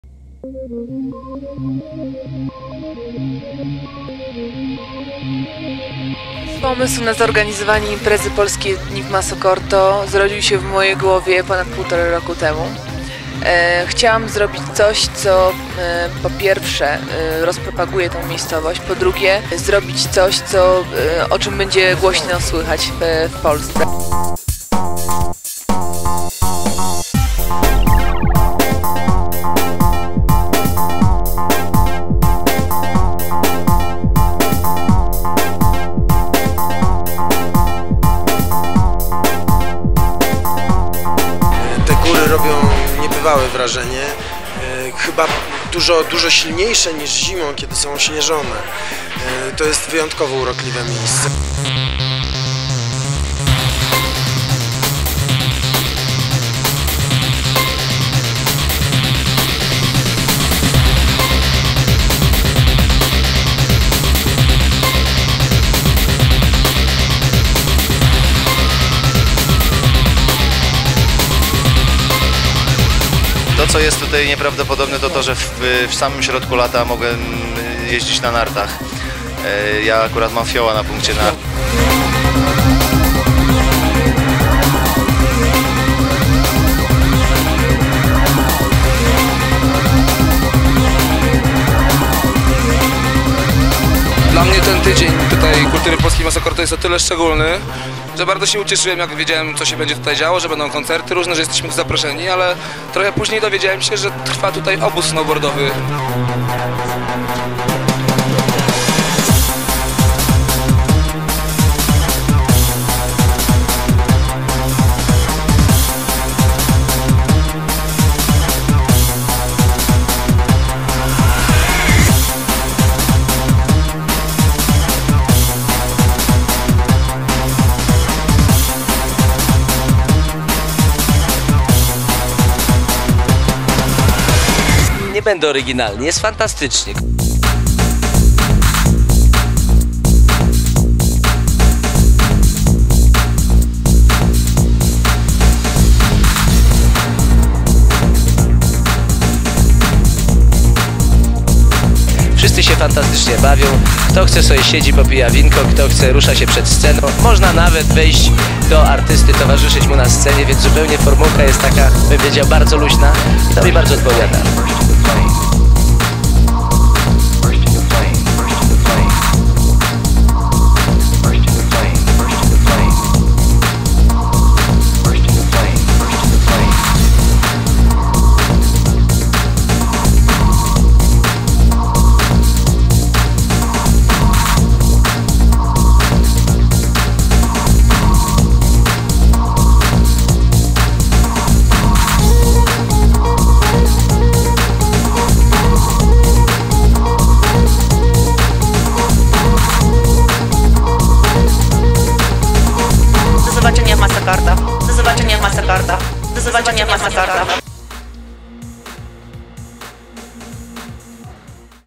Testa Communications: Maso Corto 2003
Polskie Dni w Maso Corto 2003 zrealizowane przez agencje Testa Communications.